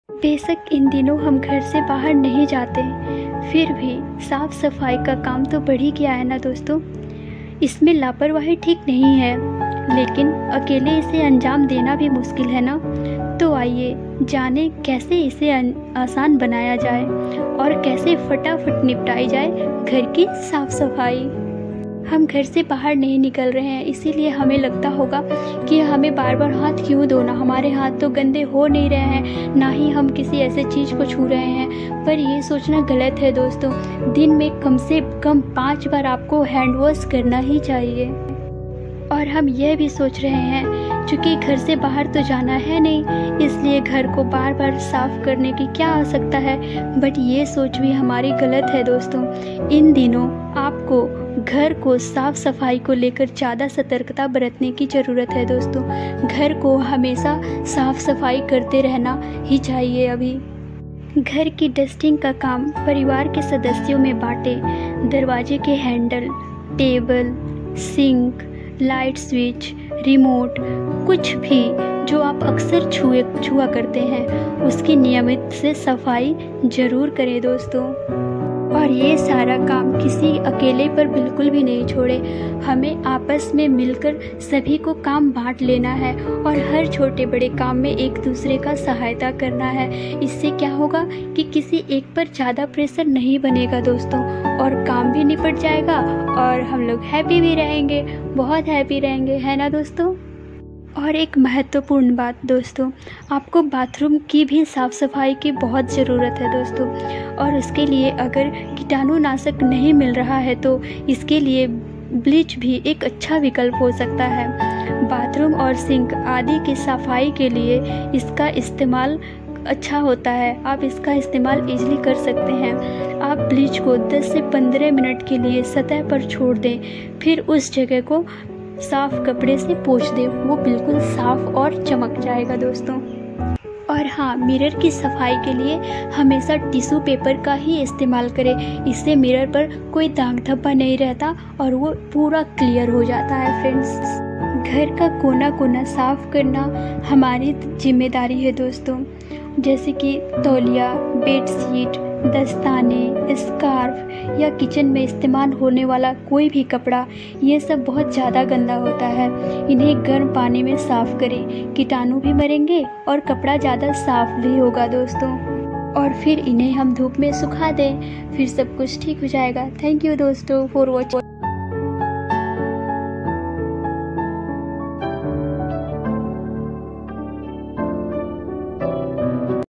इन दिनों ऐसे करें घर की साफ सफाई | family time
Besak ine dinon ham Ghar se bahar Nahin jaate fir bhi saaf safai ka kam to bade gaya hai ismein laparvahi theek Nahin lekin akele is kam ko anjam dena bhi bahut mushkil hai Tu aye Jane kaise aasan banaen jaaye aur kaise pata fat Nithya Ghar ki saaf safai

1. -. chauki bahar jana nahin hai isliye ghar ko bar bar saaf kya karna hai yah soch galat hai Hindi no aap ko ghar ko saaf safai ko lekar jyada satarkta ki jarurat hai

2;-  Ghar ki dusting ka kam parivar ke sadasyon mein baten darvaje ke handle table Singh light switch remote kuchh bhi Jo aksar chhua Jata hai uski niyamit safai jarur Karen

3:-. tolia bedsheet dastane scorpia kitchen mein istemal hone wale kapde jyada gande hote hain inhen garam Pani mein saaf Karen ki taanon bhi marenge aur kapda jyada bhi Hoga bahar aana jana ho to chappal jute ki saaf safai ka bhi khyal rakhen

4:-. Agar kitanu Na sake Ani phenyl Nahin mil raha hai to Bleach bhi achcha vikalp ho sakta hai bathroom ki safai aur Singh Kadi ki safai ke liye iska istemal bhi Karen ab likho 10 se 15 minut ke liye Safar chhod de FIR use jagah ko saaf kapde se poochh den

5:-. Ghar ke main door ke paas Hi sanitizer ka intejaam rakhen sabji vagaira laane mein istemal hone wale bags ko dhup mein sukhaye